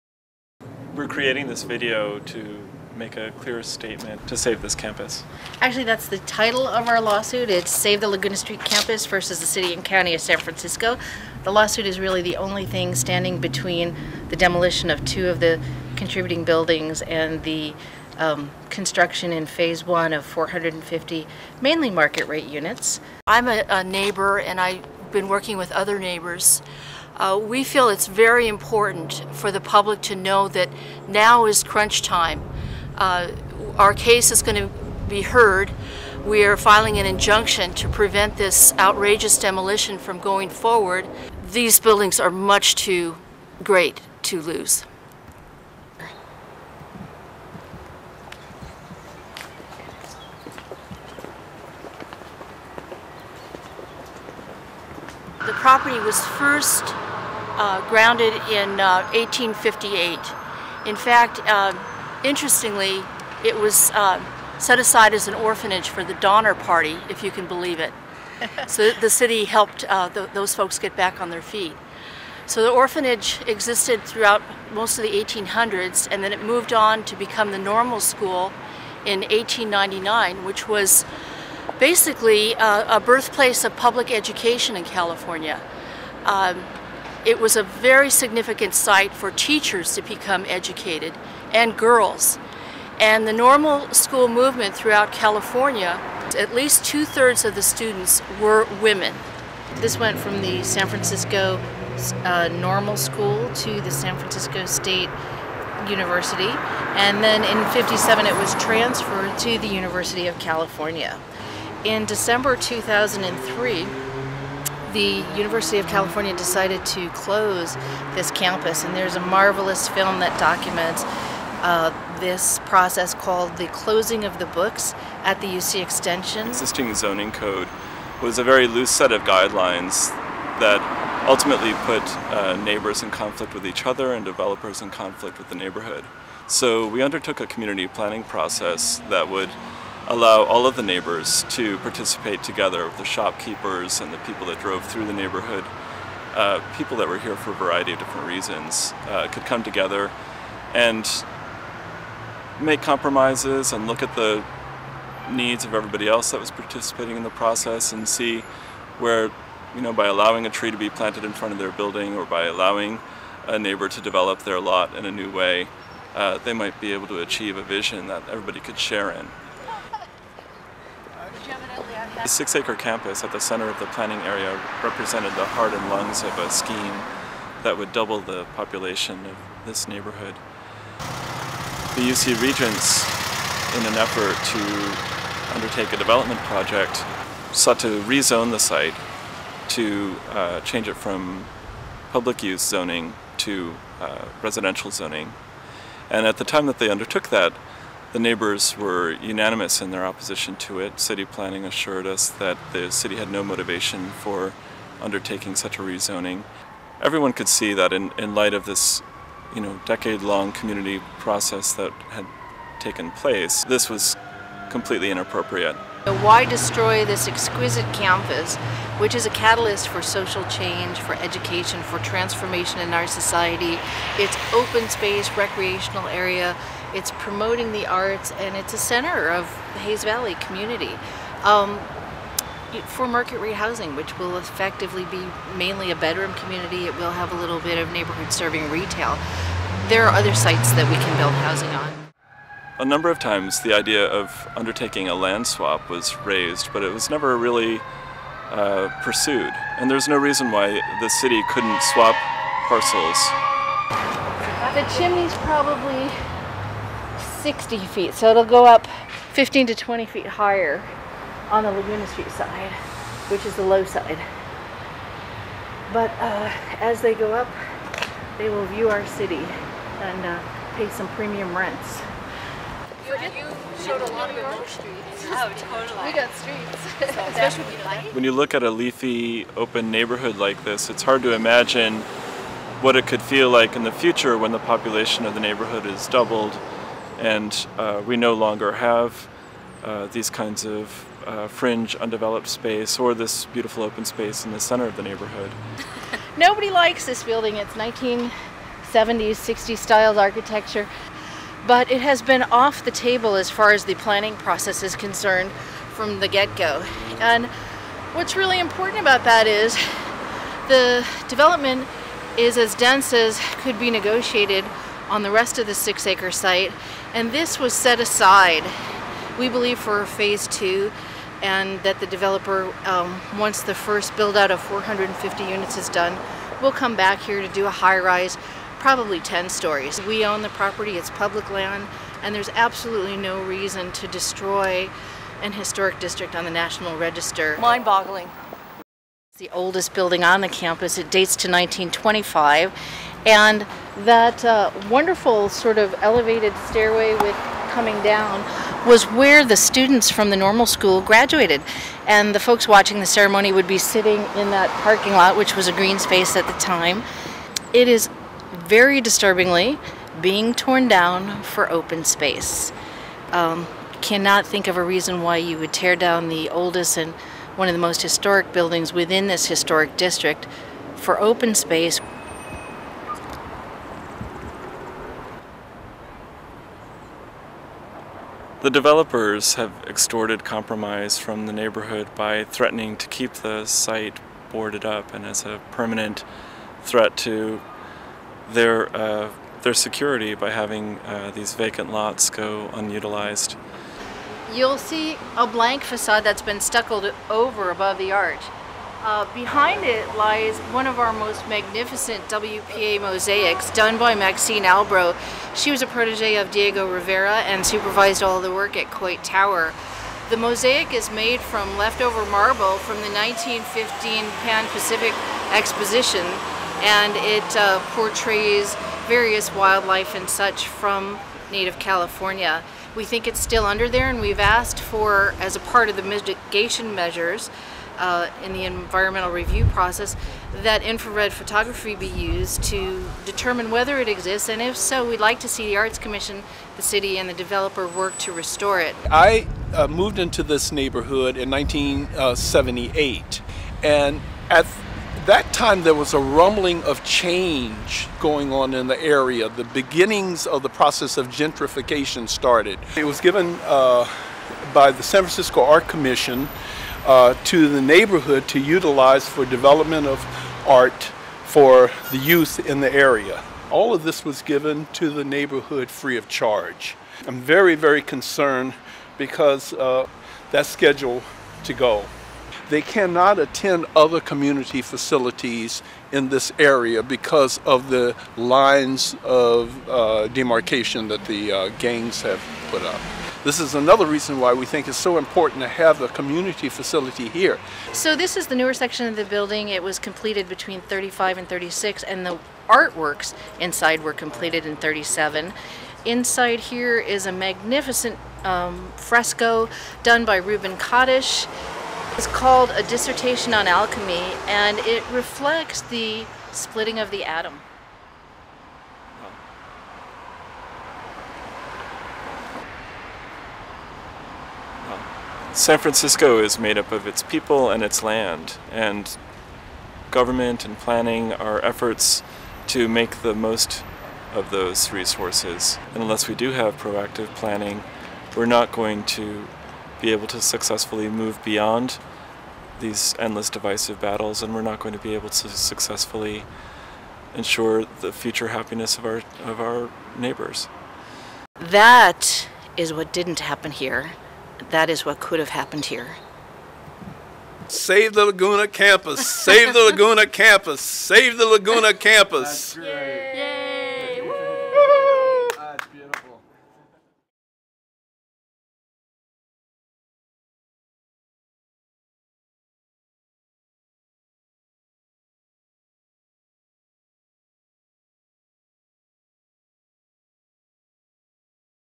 Save the Laguna Campus
Impassioned plea for reclaiming the historic Laguna Street Campus in San Francisco for it's intended community planned purpose. Site of original Protestant Orphanage Asylum, SF Normal School, etc.